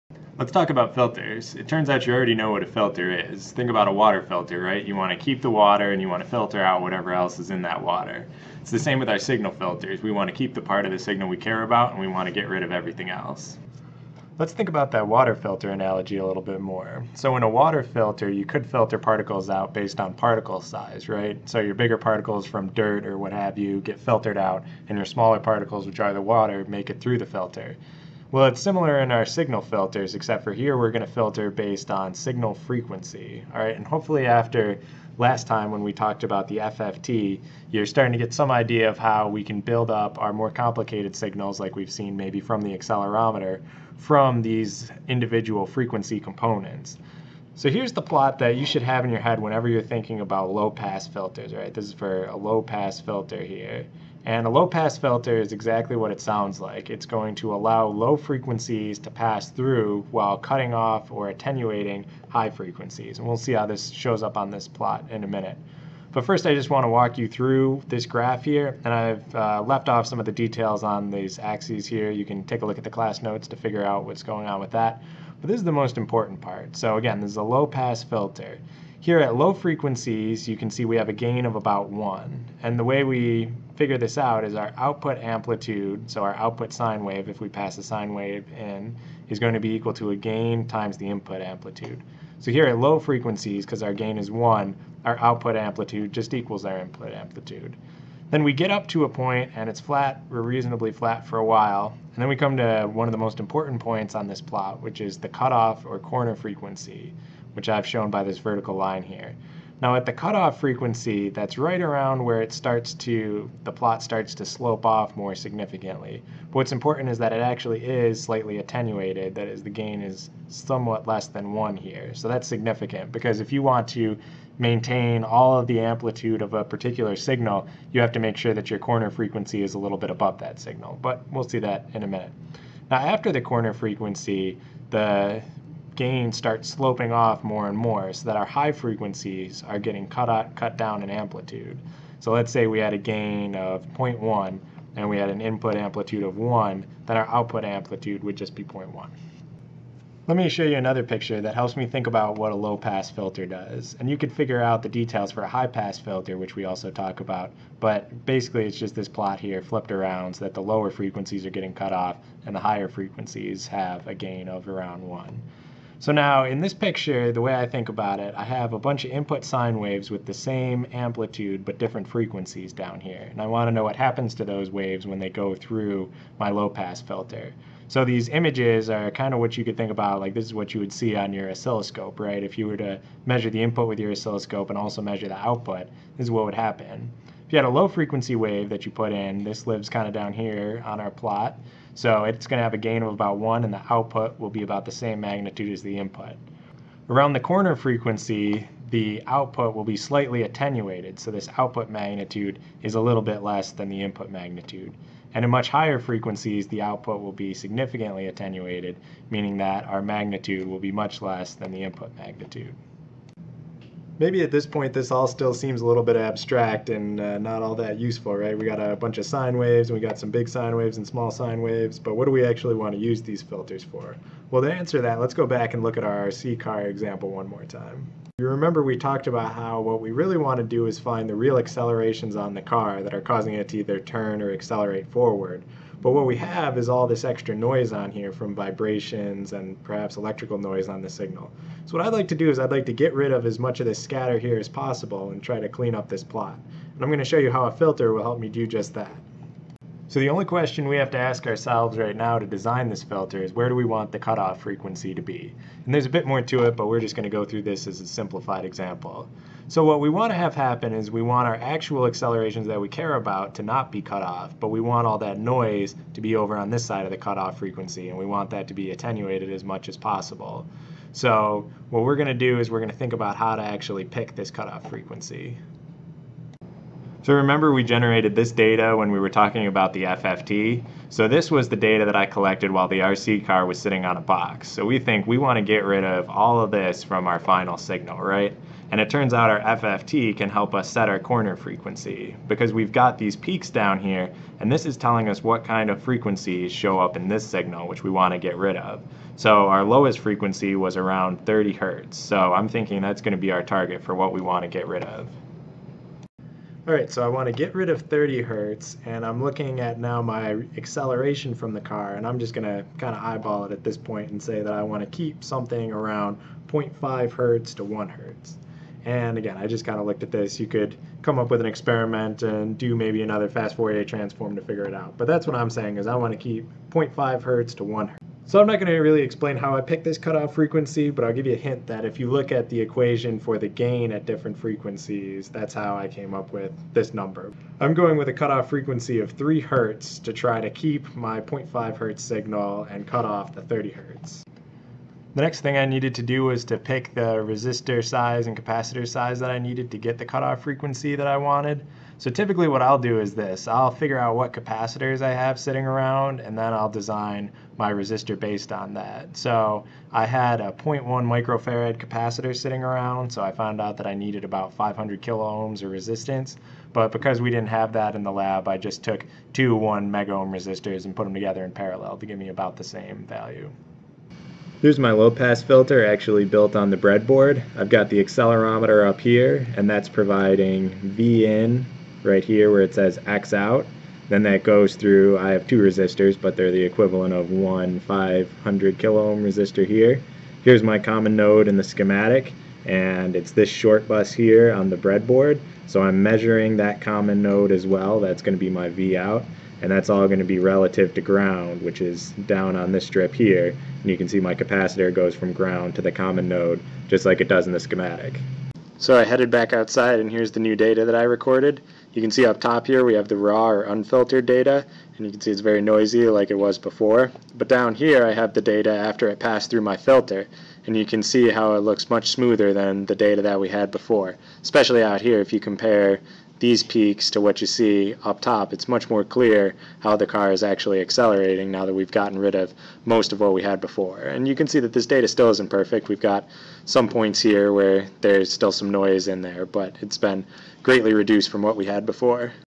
Filters
Filters are used to help reduce parts of our signal that we would like to eliminate. This video will help get you started on lab 6.